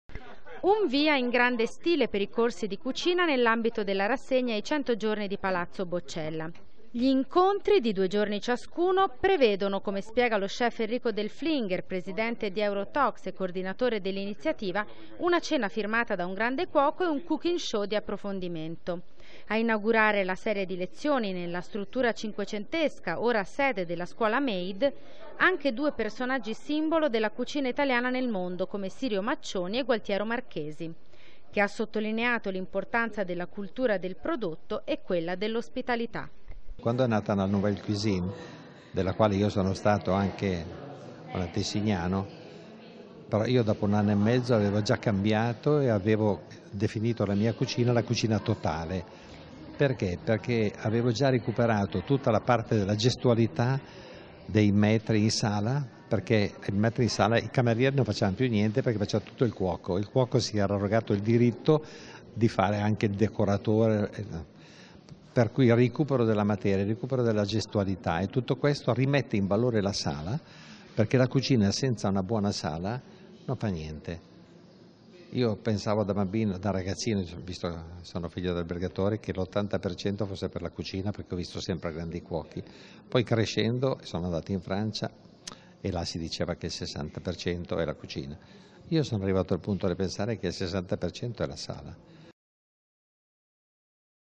Marchesi inaugura i corsi di cucina
CAPANNORI - Il maestro della cucina italiana alla giornata di lancio dei corsi di cucina nella struttura di Palazzo Boccella di San Gennaro.